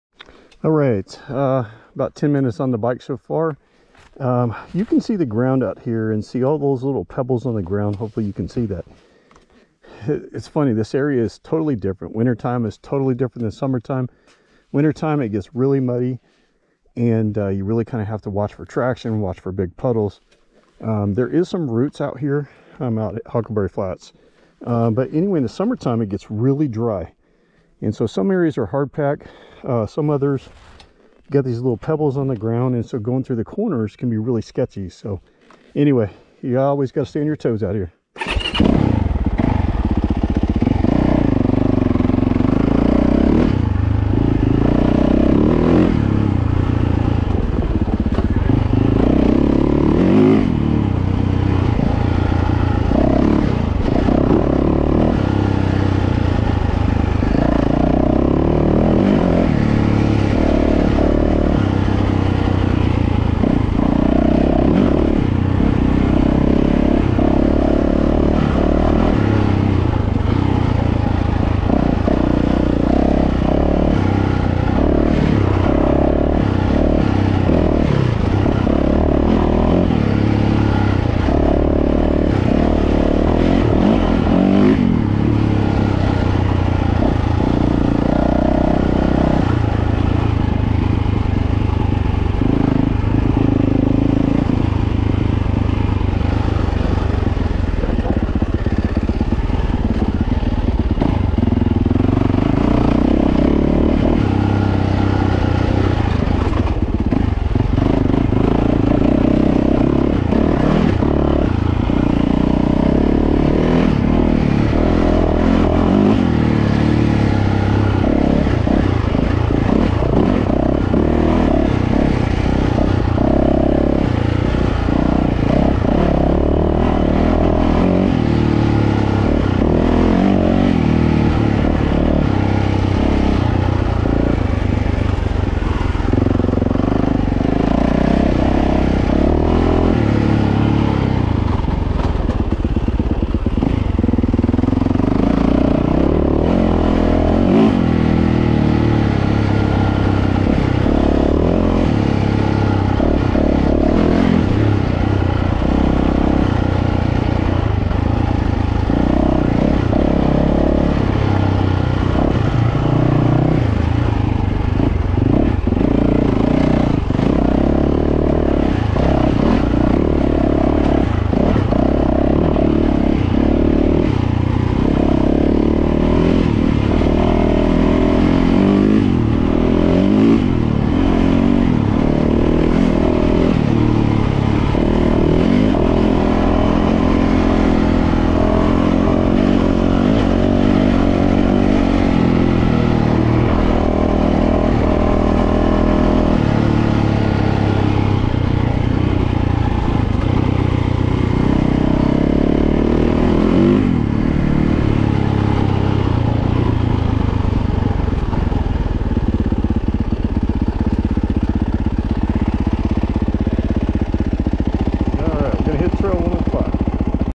Huckleberry Flats OHV part of trail 100 riding YZ450F
Riding my 2022 YZ450F on trail 100. This is a fun trail that many may not know about.